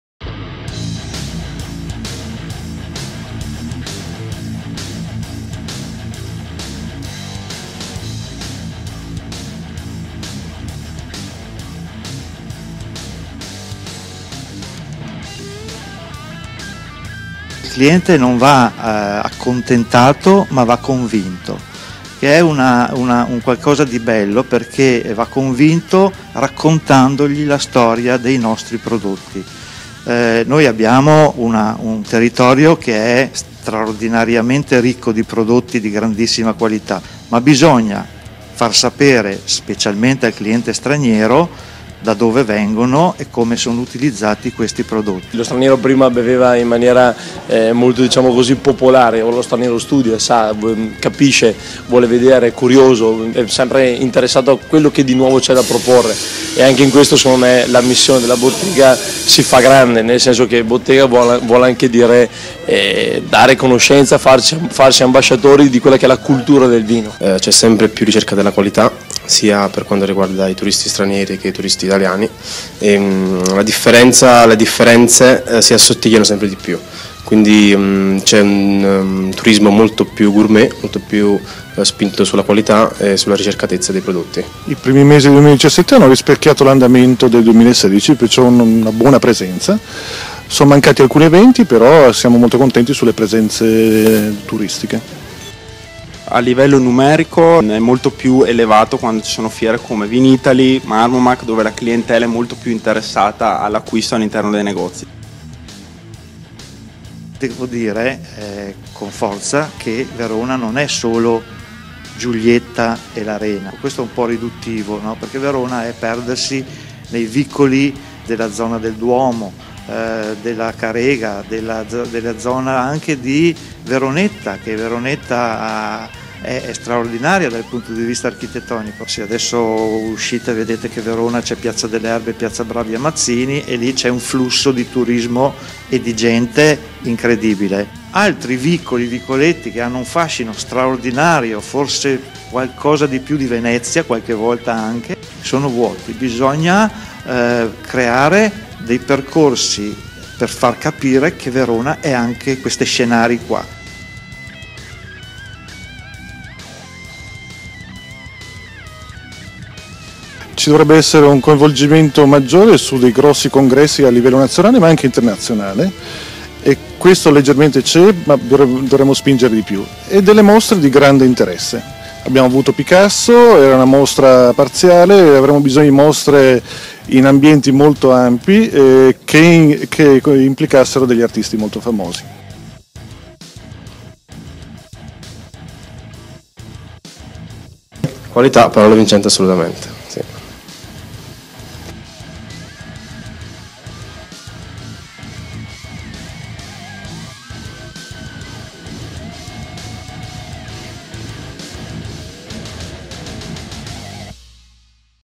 VERONESI PROTAGONISTI | VERONA TURISTICA
Numeri record registrati da VERONA nel 2016 in termini di afflusso turistico che in breve tempo hanno portato la città nella top ten italiana. Ci svelano qualche dato alcuni dei maggiori esponenti del Settore Turistico Veronese: Marco Ambrosini, Paolo Artelio, Enrico Ghinato e Marco Lucchini.